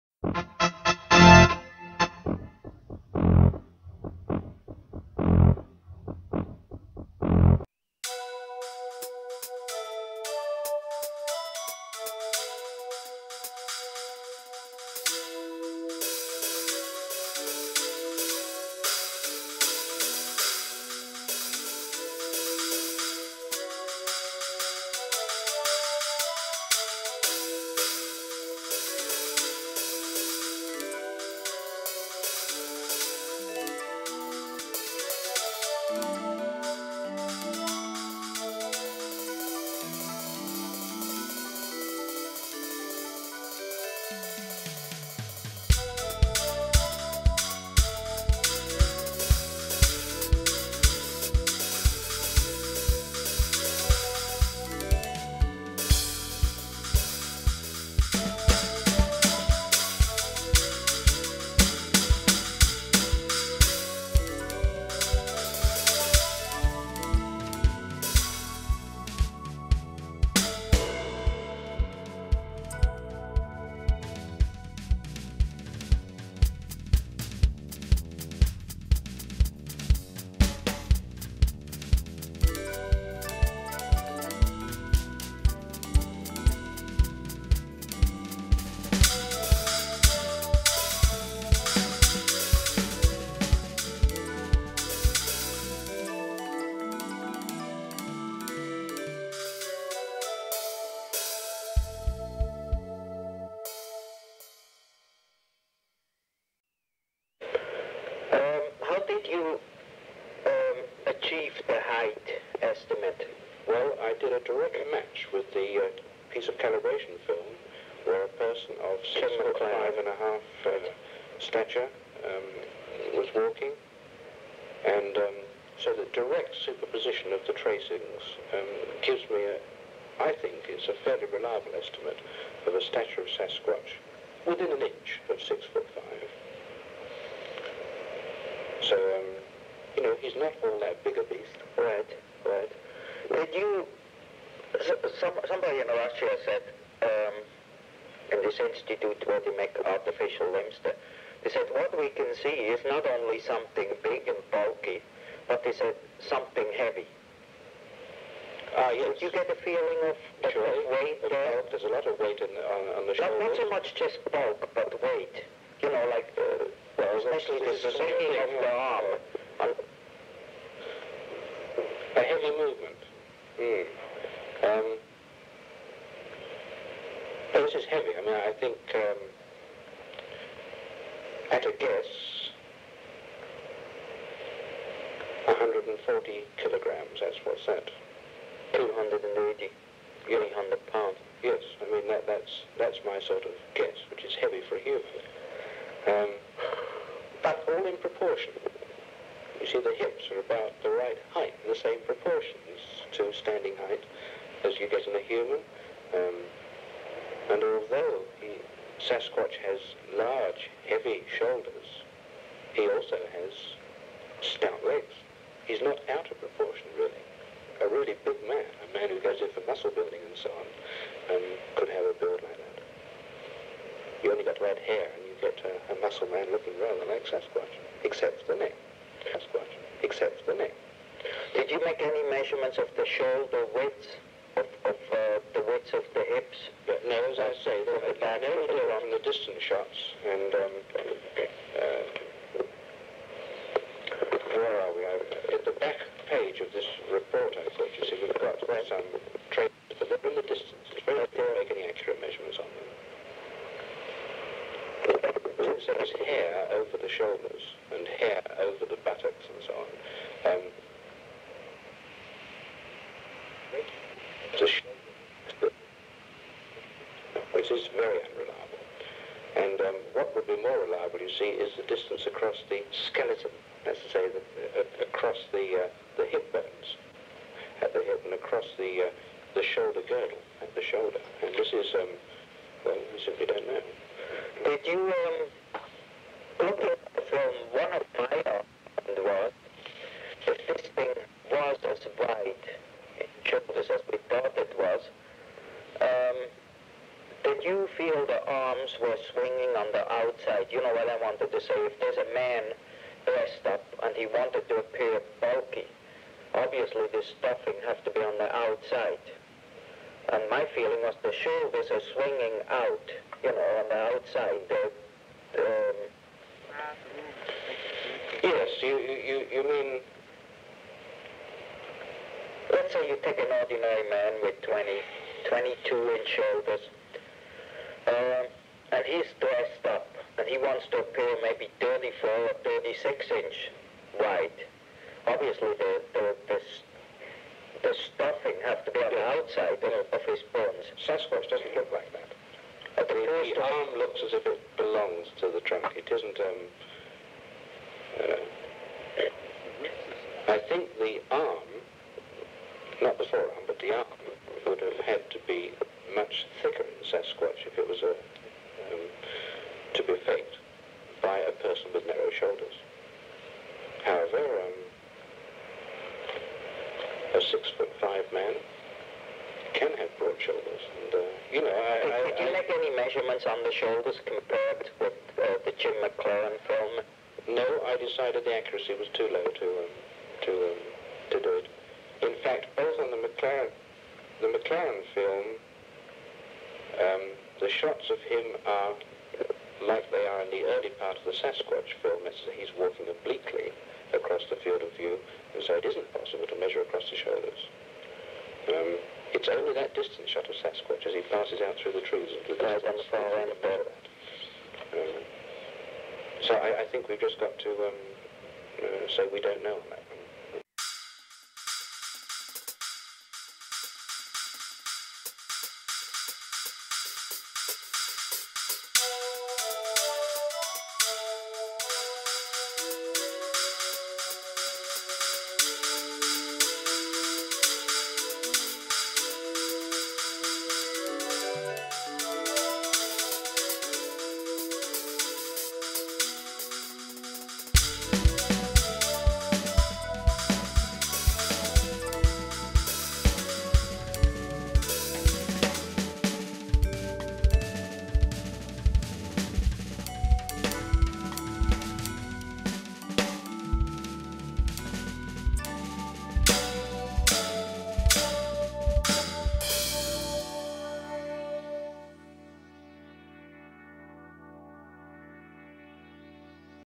René Dahinden interviewing Dr. Donald Grieve—Part 2 of 2
Dr. Donald Grieve was Professor of Biomechanics at the Royal Free Hospital School of Medicine in London and then at University College London. He was a member of the International Society of Biomechanics for many years and served on its committee, including as chair of the honours committee.

His research in occupational biomechanics and ergonomics concentrated on seeking to apply the laws of physics to the human body as a whole when exerting force.

On Friday, September 18, 2009, Dr. Grieve passed away at the age of 78. He had had a stroke in 2006, from which he never fully recovered.

Throughout his life, he was respected among his colleagues and contemporaries as a leader in the study of biomechanics.

Sasquatch researcher René Dahinden traveled to Europe in December of 1971. He met with Dr. Grieve and showed him the Patterson/Gimlin film. Grieve studied the film at length and made some very interesting and intriguing conclusions.

Thankfully, Dahinden had the foresight to record his meeting with Grieve. For the first time ever, the audio from this meeting is available to the public.

It must be stated that without John Green’s tenacity in archiving materials such as the Grieve interview, some of these old audio clips would be lost forever. So, gratitude is extended to John Green (1927–2016) for his life-long and tireless efforts.

The video and music are inherently copyrighted—but if you wish to share or re-post, I kindly request that you ask for permission and proper credit be given.

Thank-you for watching and I hope you enjoy it!
TODD PRESCOTT